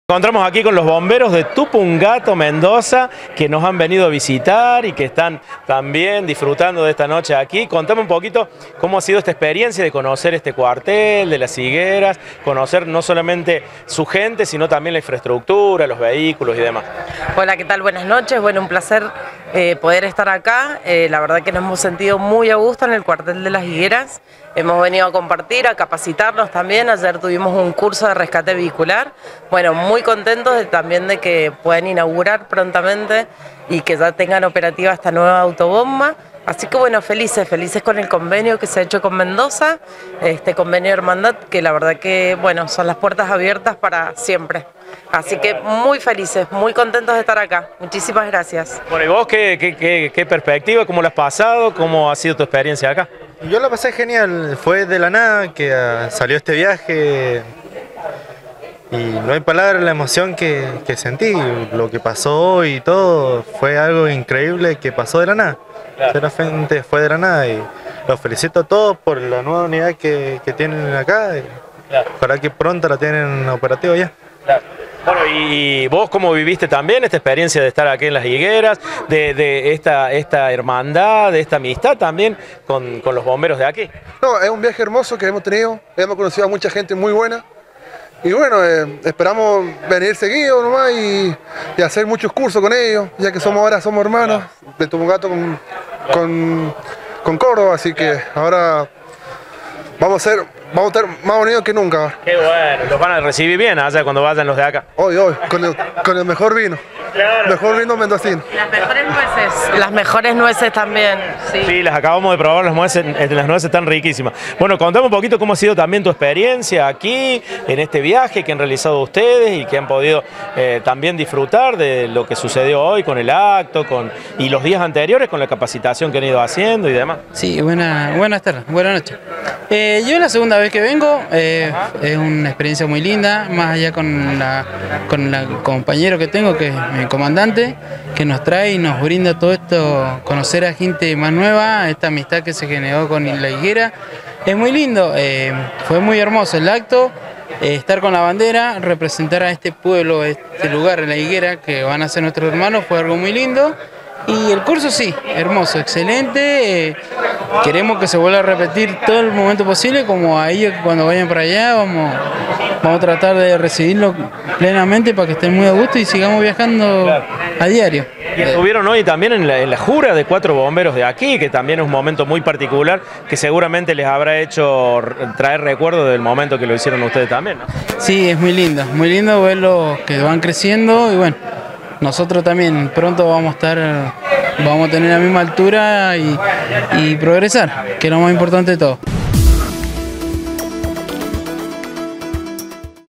Bomberos Voluntarios de Tupungato Mendoza realizaron un curso de capacitación en Las Higueras
Bomberos Voluntarios de Tupungato Mendoza realizaron curso de capacitación en Las Higueras

Además participaron del acto de asunción de 4 nuevos Bomberos Voluntarios de Las Higueras y de la presentación de un nuevo móvil del caurtel 165.

Durante el acto, el Intendente Alberto Escudero saludó a Carlos Félix del cuatel de Bomberos Voluntarios de Tupungato Maendoza y Roberto Cugino hizo entrega de una copia del convenio de hermandad Bomberil entre el cuartel mendocino y el de Las Higueras, que les ha permitido realizar capacitaciones, cursos y maniobras de entrenamiento en conjunto.